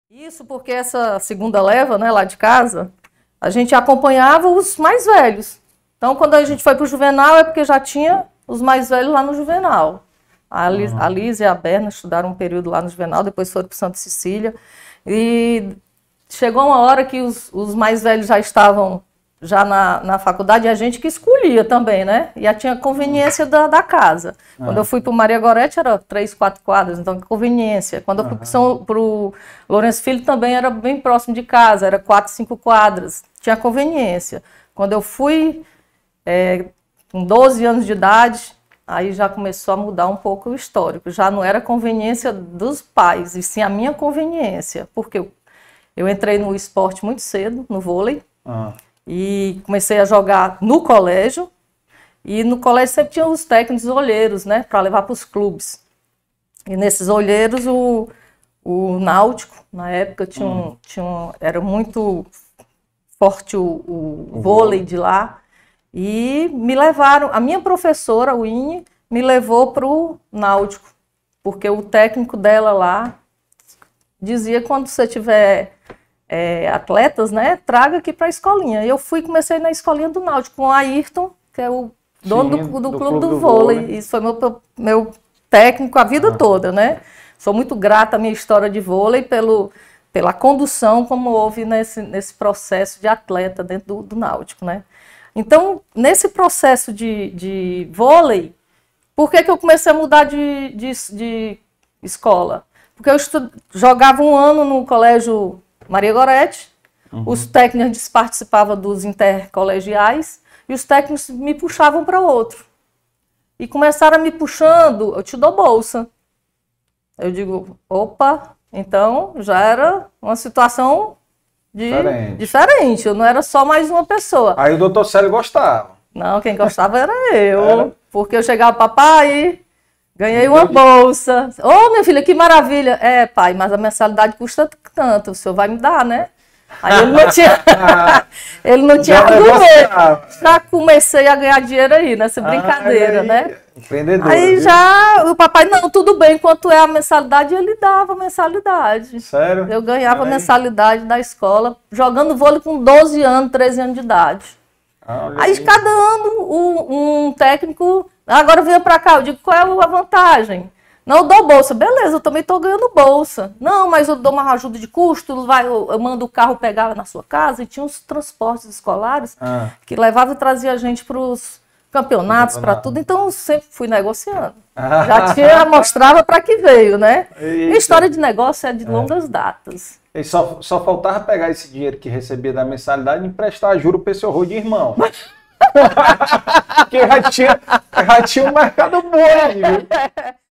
FUI ATLETA DE VOLEI POR MUITO TEMPO NO ENSINO MEDIO! FALA SILENE GURGEL FUNDADORA FAZENDINHA
DEI VALOR PODCAST EP. 178 - SILENE GURGEL - EMPRESÁRIA